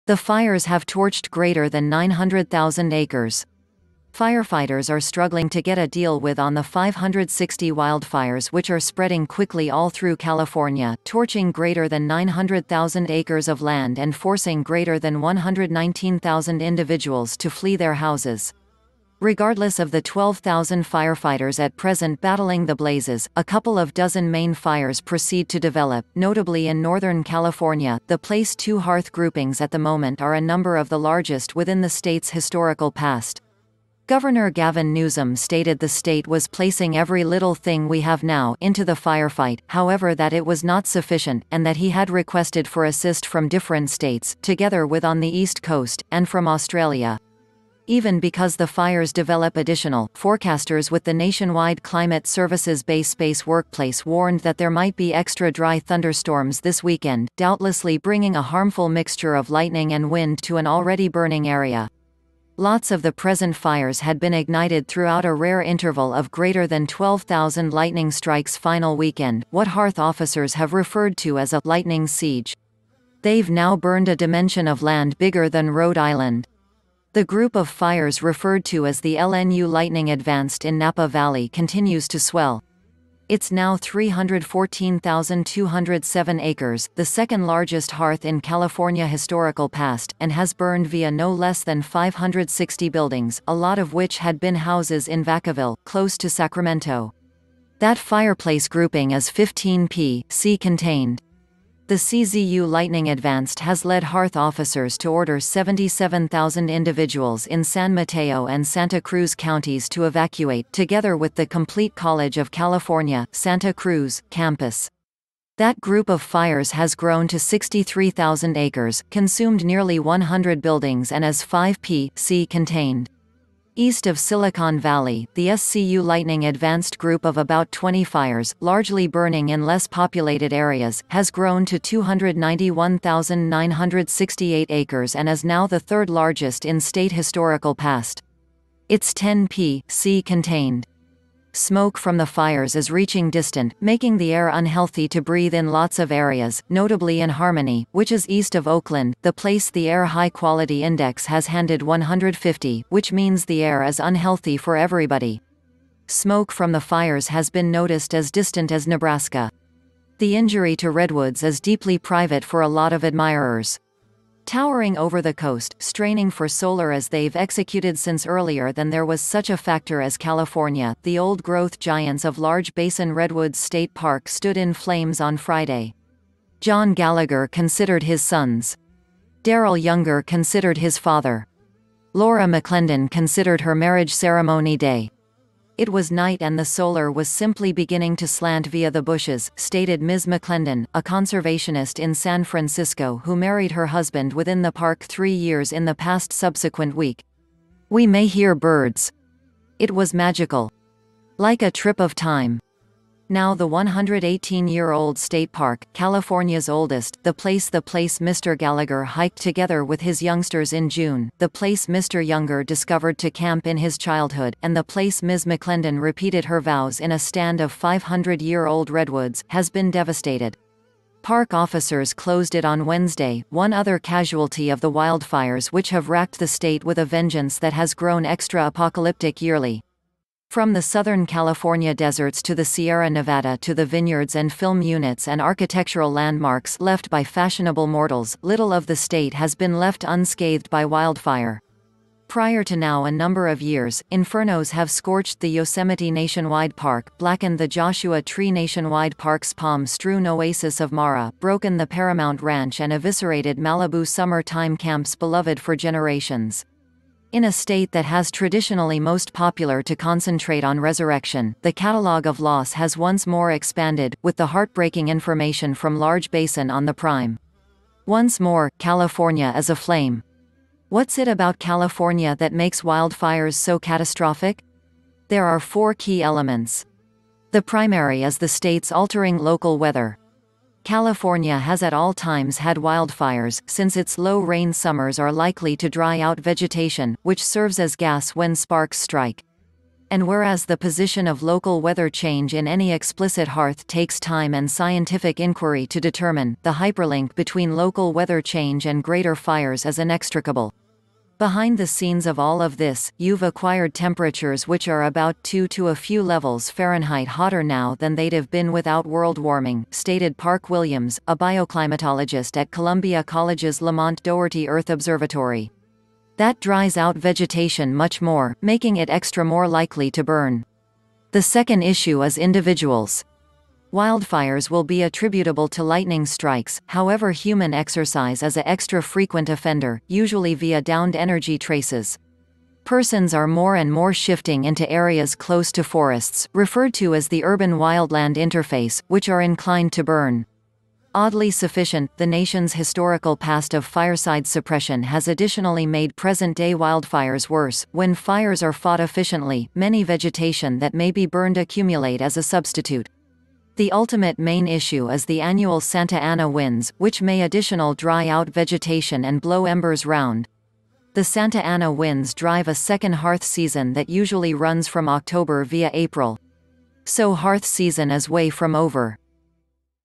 Dwell Updates of the California Wildfires
The fires have torched greater than 900,000 acres.
Firefighters are struggling to get a deal with on the 560 wildfires which are spreading quickly all through California, torching greater than 900,000 acres of land and forcing greater than 119,000 individuals to flee their houses.

Regardless of the 12,000 firefighters at present battling the blazes, a couple of dozen main fires proceed to develop, notably in Northern California, the place two hearth groupings at the moment are a number of the largest within the state’s historical past. Gov. Gavin Newsom stated the state was “placing every little thing we have now” into the firefight, however that it was not sufficient, and that he had requested for assist from different states — together with on the East Coast — and from Australia.

Even because the fires develop additional, forecasters with the Nationwide Climate Service’s Bay Space workplace warned that there might be extra dry thunderstorms this weekend, doubtlessly bringing a harmful mixture of lightning and wind to an already-burning area. Lots of the present fires had been ignited throughout a rare interval of greater than 12,000 lightning strikes final weekend, what hearth officers have referred to as a “lightning siege.” They’ve now burned a dimension of land bigger than Rhode Island.

The group of fires referred to as the L.N.U. Lightning Advanced in Napa Valley continues to swell. It’s now 314,207 acres — the second-largest hearth in California historical past — and has burned via no less than 560 buildings, a lot of which had been houses in Vacaville, close to Sacramento. That fireplace grouping is 15 p.c contained.

The C.Z.U. Lightning Advanced has led hearth officers to order 77,000 individuals in San Mateo and Santa Cruz Counties to evacuate, together with the complete College of California, Santa Cruz, campus. That group of fires has grown to 63,000 acres, consumed nearly 100 buildings and is 5 p.c contained.

East of Silicon Valley, the S.C.U. Lightning Advanced group of about 20 fires — largely burning in less-populated areas — has grown to 291,968 acres and is now the third-largest in state historical past. It’s 10 p.c contained.

Smoke from the fires is reaching distant, making the air unhealthy to breathe in lots of areas, notably in Harmony, which is east of Oakland, the place the air high quality index has handed 150, which means the air is unhealthy for everybody. Smoke from the fires has been noticed as distant as Nebraska.

The injury to redwoods is deeply private for a lot of admirers.
Towering over the coast, straining for solar as they’ve executed since earlier than there was such a factor as California, the old-growth giants of Large Basin Redwoods State Park stood in flames on Friday. John Gallagher considered his sons. Darryl Younger considered his father. Laura McLendon considered her marriage ceremony day.

“It was night and the solar was simply beginning to slant via the bushes,” stated Ms. McLendon, a conservationist in San Francisco who married her husband within the park three years in the past subsequent week. “We may hear birds. It was magical. Like a trip of time.”